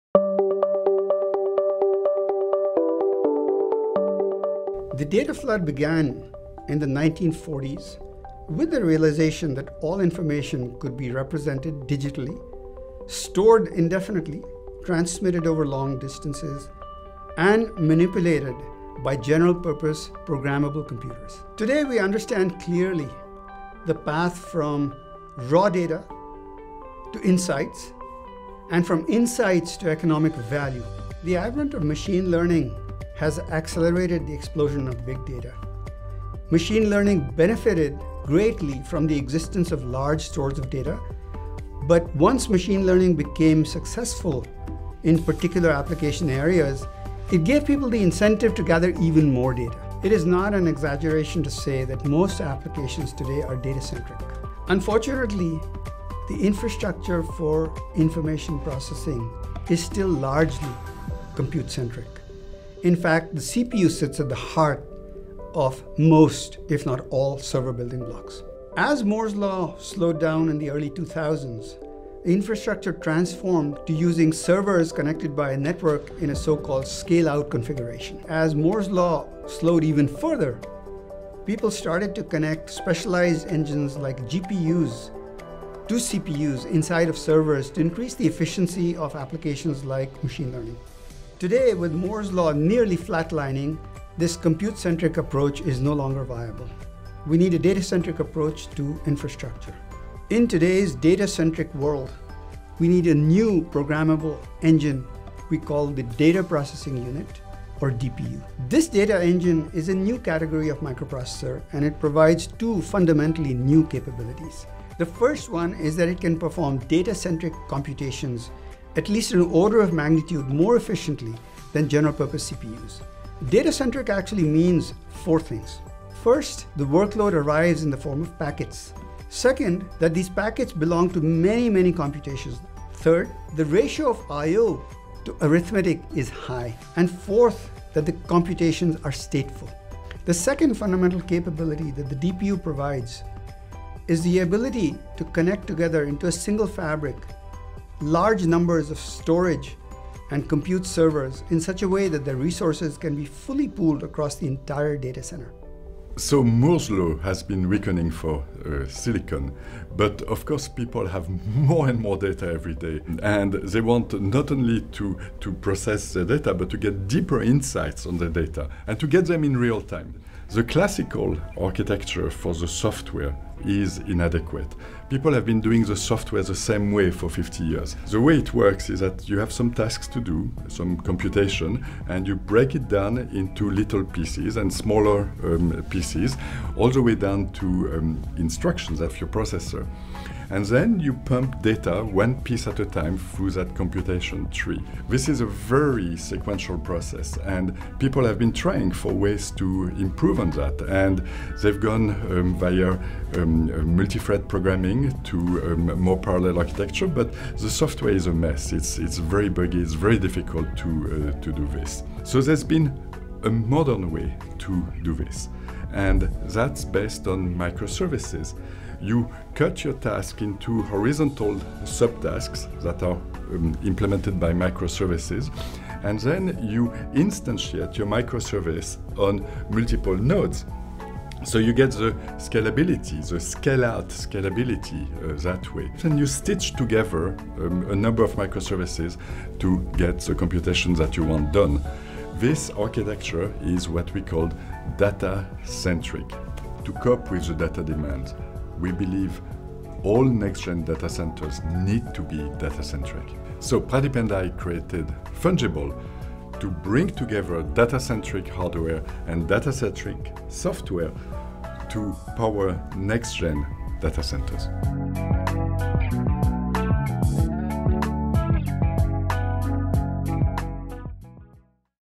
What Do Next-Generation Data Centers Look Like?
Today's data centers are tasked with processing colossal amounts of data every day. Are they equipped to keep up? In this video, Fungible founders Pradeep Sindhu and Bertrand Serlet discuss how data centers have changed over the years and what next-generation data center architecture should look like.

The data flood began in the 1940s with the realization that all information could be represented digitally stored indefinitely transmitted over long distances and manipulated by general-purpose programmable computers. Today we understand clearly the path from raw data to insights and from insights to economic value.
The advent of machine learning has accelerated the explosion of big data. Machine learning benefited greatly from the existence of large stores of data, but once machine learning became successful in particular application areas it gave people the incentive to gather even more data. It is not an exaggeration to say that most applications today are data-centric. Unfortunately, the infrastructure for information processing is still largely compute-centric. In fact,– in fact, the CPU sits at the heart of most if not all server building blocks.
As Moore's Law slowed down in the early 2000s, infrastructure transformed to using servers connected by a network in a so-called, "scale-out configuration." As Moore's Law slowed even further, people started to connect specialized engines like GPUs to CPUs inside of servers to increase the efficiency of applications like machine learning. Today with Moore's Law nearly flatlining, this compute-centric approach is no longer viable.
 We need a data-centric approach to infrastructure. In today's data-centric world, we need a new programmable engine we call the data processing unit, or DPU. This data engine, is a new category of microprocessors and it provides two fundamentally new capabilities. The first one is that it can perform data-centric computations at least an order of magnitude more efficiently than general-purpose CPUs. Data-centric actually means four things: first, the workload arrives in the form of packets. Second, that these packets belong to many, many computations. Third, the ratio of IO to arithmetic is high, and fourth, that the computations are stateful. The second fundamental capability that the DPU provides is the ability to connect together into a single fabric large numbers of storage and compute servers in such a way that their resources can be fully pooled across the entire data center. So Moore's Law has been weakening for silicon, but of course people have more and more data everyday and they want not only to process that data, but to get deeper insights on that data and to get them in real-time. That data and to get them in real-time.
The classical architecture for the software is inadequate – people have been doing the software the same way for fifty years. The way it works, is that you have some tasks to do, some computations, and you break it down into little pieces, and smaller pieces, all the way down to instructions of your processor and then you pump data one piece at a time through that computation tree. This is a very sequential process and people have been trying for ways to improve on that down into little pieces, and smaller pieces, all the way down to instructions of your processor and then you pump data one piece at a time through that computation tree. This is a very sequential process and people have been trying for ways to improve on that and they've gone via multi-thread programming to more parallel architecture, the software is a mess it's very buggy – it's very difficult to do this. So, there's been a modern way to do this – and that's based on microservices. You cut your task into horizontal subtasks that are implemented by microservices, and then you instantiate your microservice on multiple nodes, so you get the scalability, the "scale-out" scalability that way. Then, you stitch together a number of microservices to get the computation that you want done. This architecture is what we call data-centric. To cope with the data demands, we believe all next-gen data centers need to be data-centric. So, Pradeep and I created Fungible, to bring together data-centric hardware and data-centric software to power next-gen data centers.